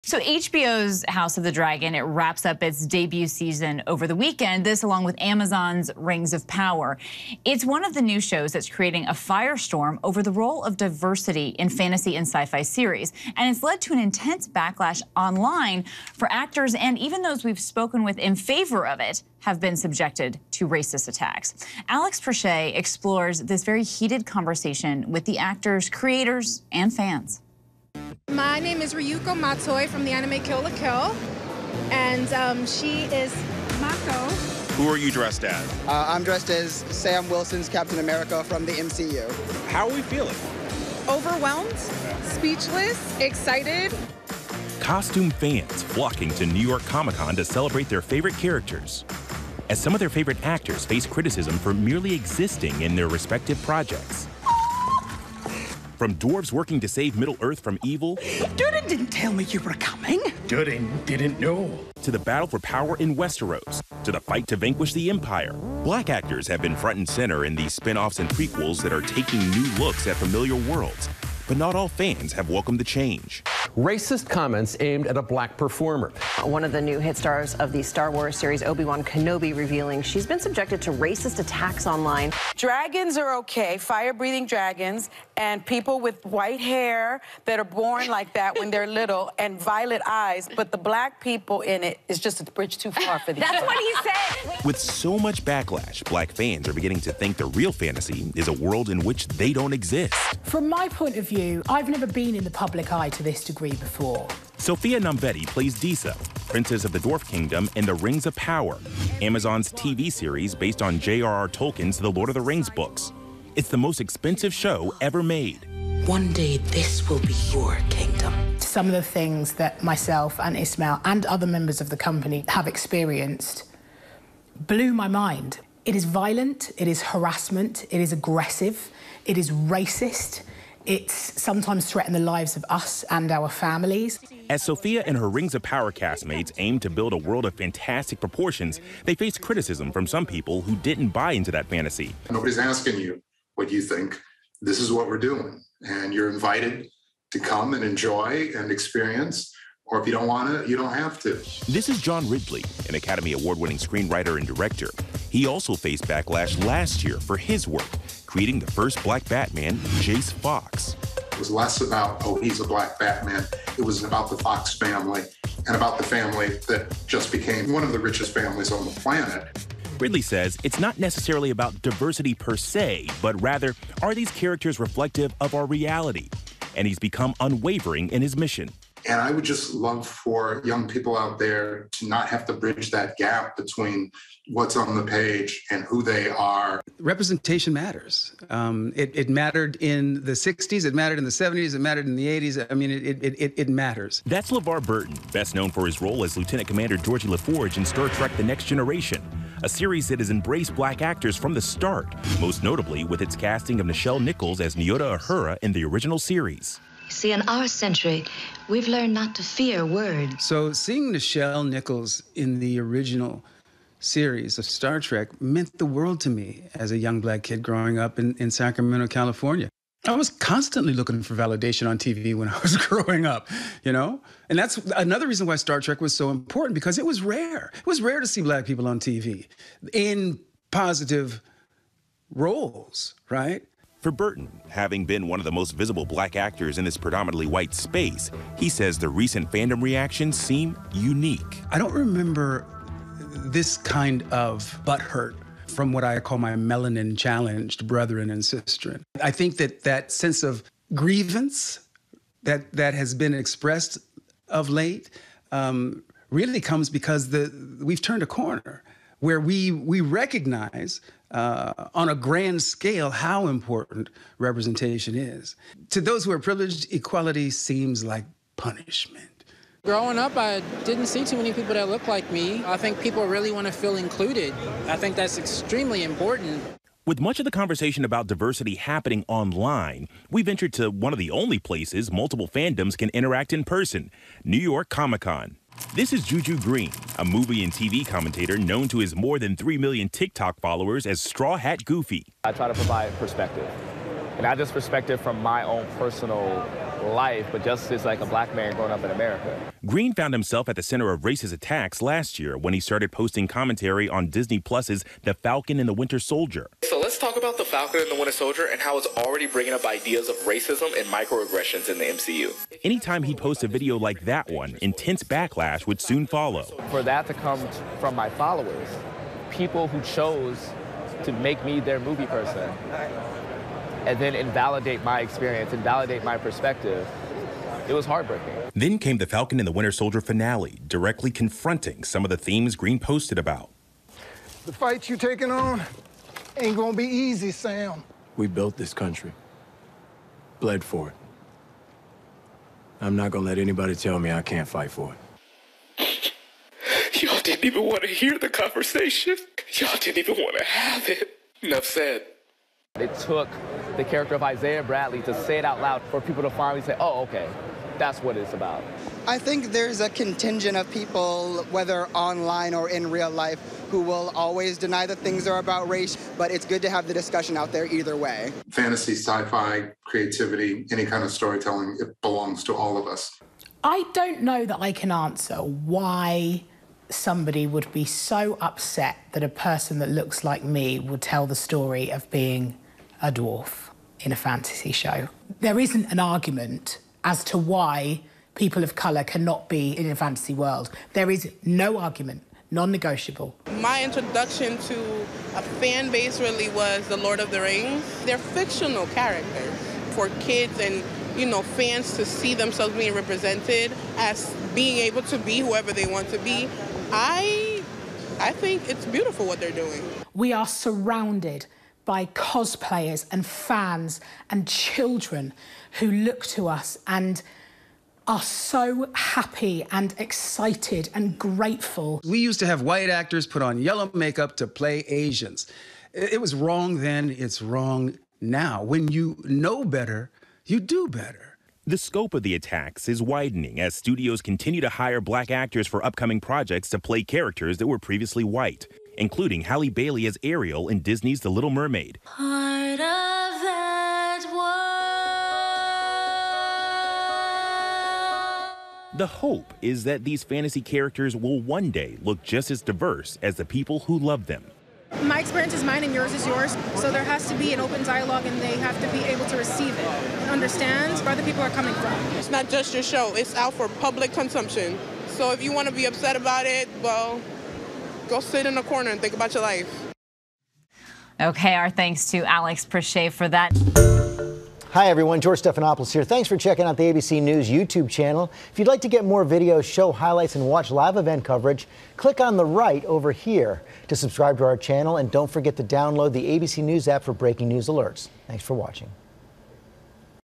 'Representation matters': Firestorm over Black actors in sci-fi, fantasy shows | ABCNL
ABC News' Alex Presha reports on actors, creators and fans reacting to the backlash and racist attacks over the inclusion of Black actors in sci-fi/fantasy series, films and comic books.